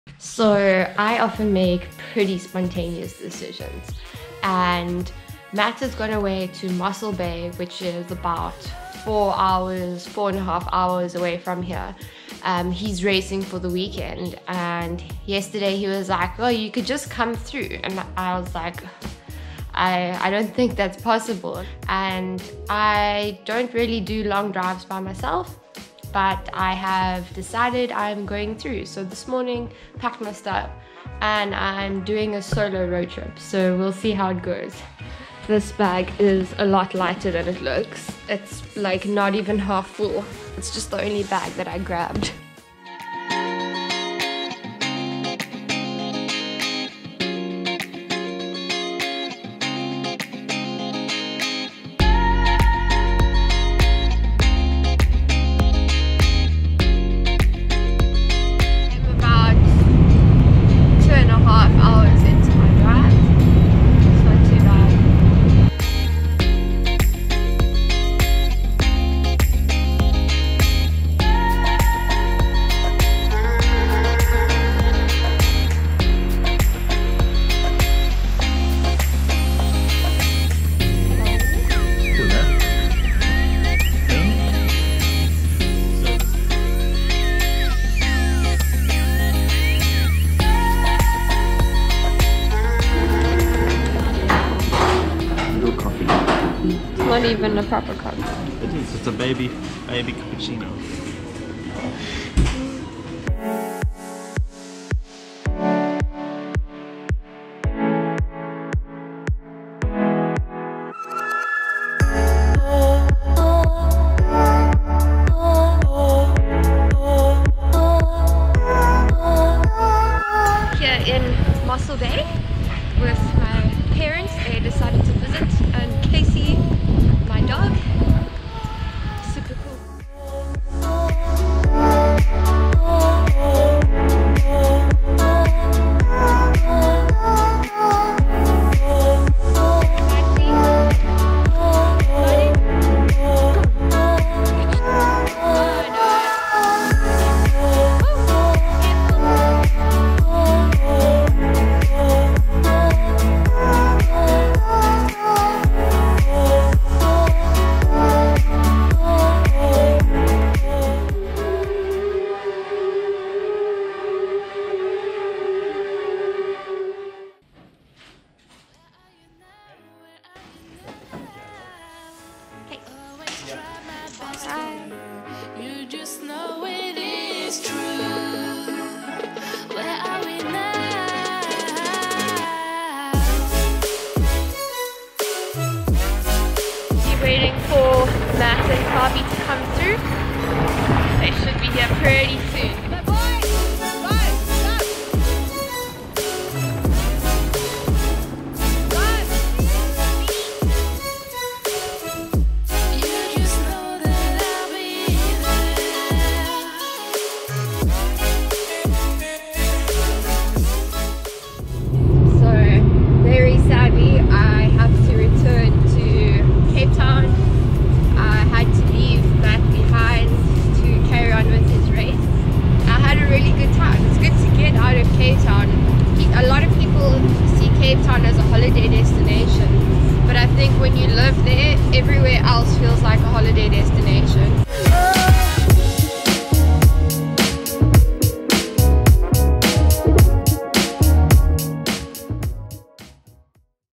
My first 'Vlog' EVER!
Went on a spontaneous adventure and decided to document part of it using the GoPro HERO7 and try something a bit different.

Matt went away to race at Cape Pioneer Trek and I decided to take a four and a half hour road trip to join him for the weekend.